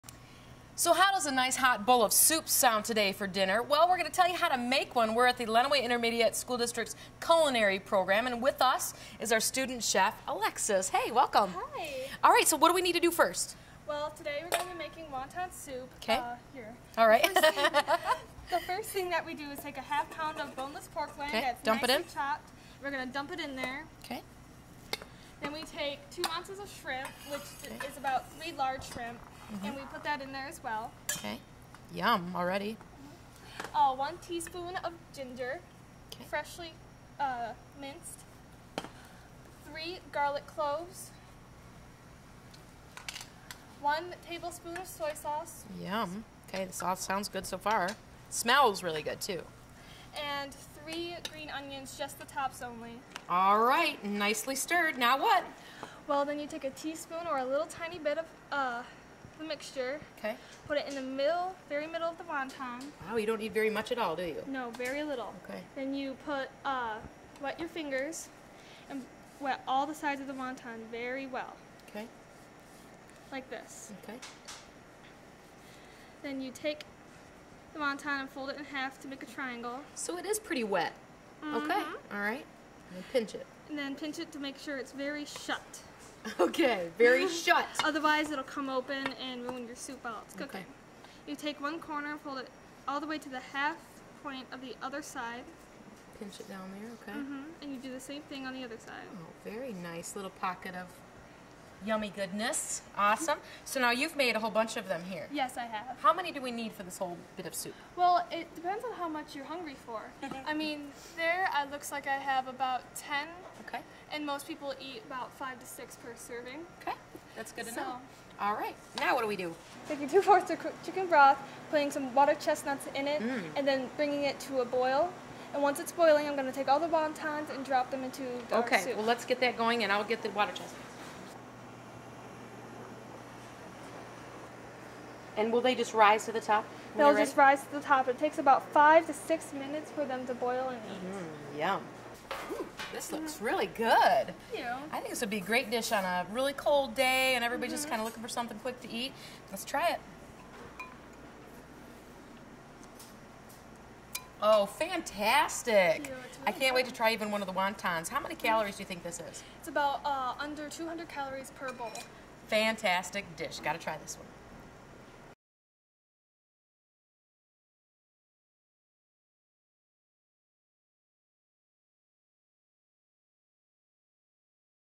Easy Won Ton Soup
Try this easy recipe for Won Ton Soup from the student chefs at Lenawee Intermediate School Distict's Hospitality and Culinary class. Kelly Heidbreder and the student chefs make it look easy and delicious!